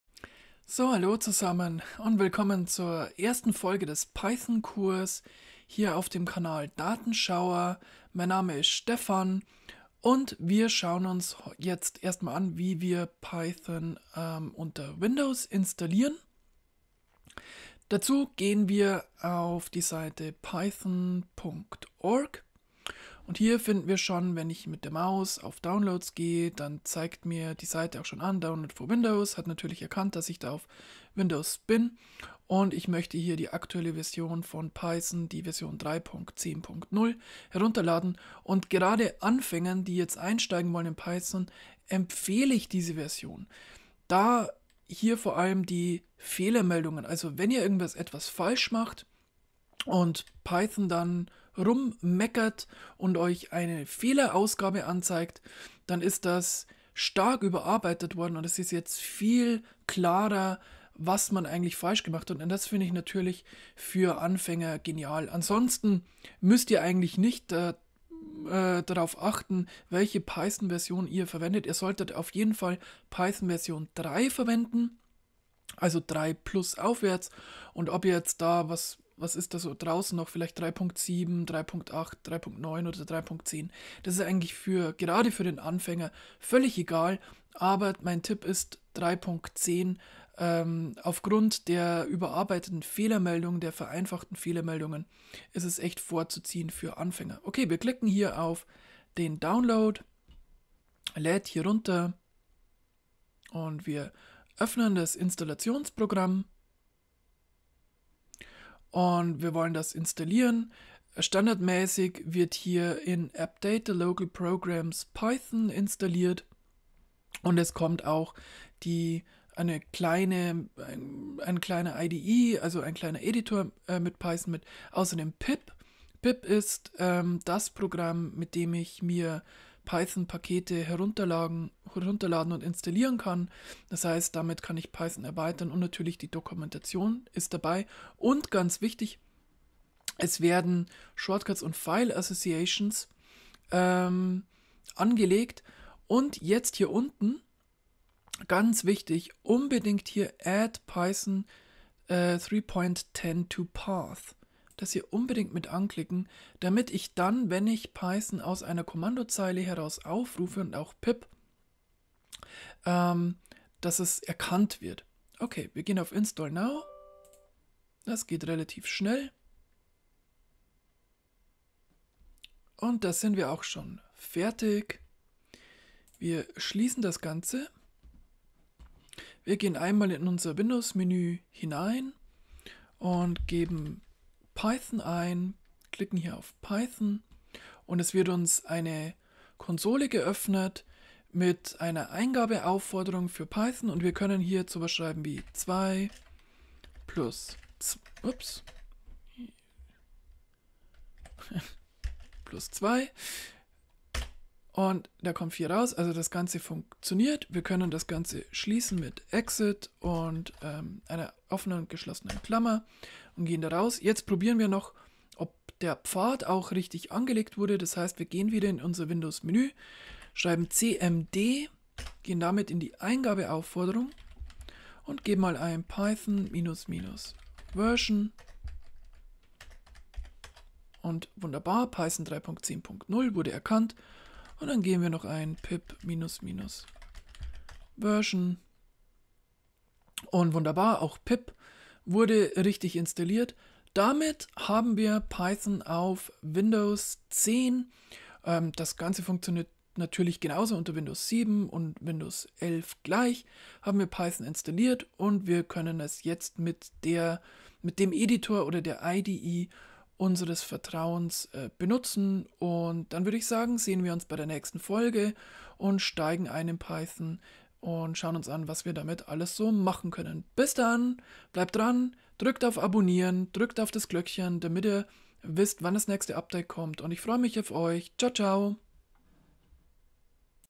Python Kurs #1: Installation unter Windows 10
In diesem ersten Video zeige ich euch, wie ihr Python unter Windows 10 installiert.